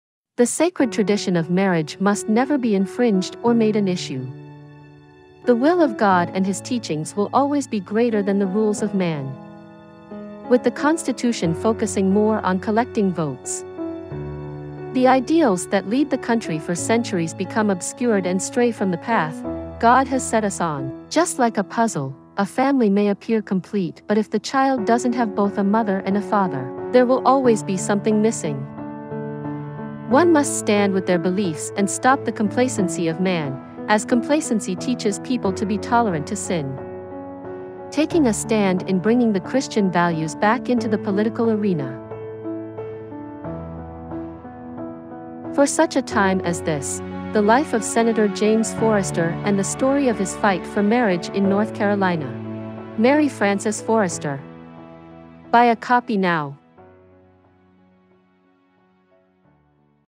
For Such a Time as This: The Life of Senator James Forrester A | Mary Frances Forrester
Book Description:
For Such a Time as This is a must-read for anyone disheartened over America's slide into a moral "cesspool of sin." The compelling story of Jim Forrester's life and his fight for traditional marriage are an indispensable legacy to the next generation of Americans who wish to preserve the nuclear family and all other Judeo-Christian bedrocks.

Here is a poignant example of how to face opposition, both personal and public, with courage, determination, and grace. This book will fan the flame of resistance and renew hope within the heart of every reader. Can we claw our way back up the slippery slope?

Product Details:
Publisher ‏ : ‎ Trilogy Christian Publishing (January 14, 2021)
Language ‏ : ‎ English
Paperback ‏ : ‎ 408 pages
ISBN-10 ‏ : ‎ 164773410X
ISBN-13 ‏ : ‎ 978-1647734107
Item Weight ‏ : ‎ 1.31 pounds
Dimensions ‏ : ‎ 6 x 0.91 x 9 inches